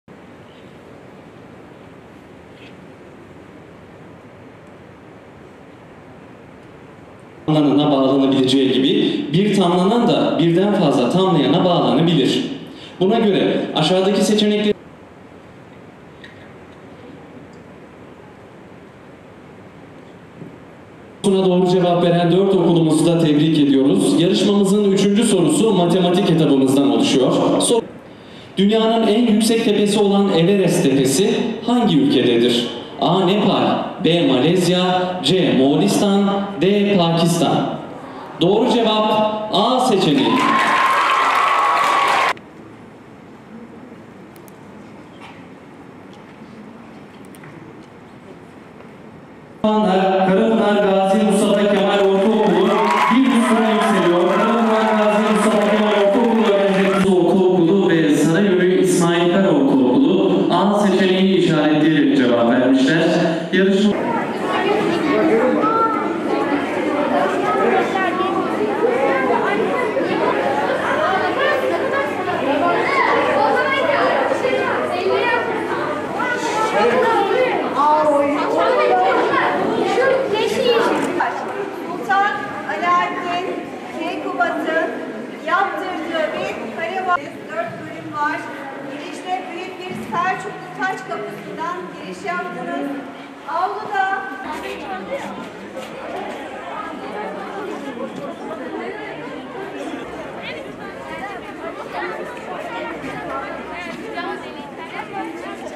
“Haydi Bil Bakalım” Yarışması Konya Finalleri
Konya Büyükşehir Belediyesi ve İl Milli Eğitim Müdürlüğü ortaklığıyla şehir genelindeki ortaokullara yönelik gerçekleştirilen “Haydi Bil Bakalım” adlı bilgi yarışmasının Konya elemeleri büyük heyecana sahne oluyor.

Konya Büyükşehir Belediyesi’nin Medeniyet Okulu Projesi kapsamında düzenlediği “Haydi Bil Bakalım” bilgi yarışmasının Konya elemeleri devam ediyor. Büyükşehir Belediyesi Mevlana Kültür Merkezi’ndeki elemelere 31 ilçedeki ortaokullar katılırken ilçelerinde birinci olarak Konya il finallerinde yarışmaya hak kazanan okullar, bir üst tura çıkabilmek için bütün hünerlerini sergiliyor. Büyük heyecana sahne olan il elemelerinde kendi aralarında yeniden yarışan ilçe birincileri, bilgi yarışmasının Mayıs ayında yapılacak büyük finalinde bu kez Konya birincisi olabilmek için mücadele verecek. İl Milli Eğitim Müdürlüğü ortaklığıyla gerçekleştirilen organizasyonda, Konya elemeleri için ilçelerden merkeze gelen öğrenciler, şehrin tarihi ve turistik mekanlarını da gezme imkanı buluyor.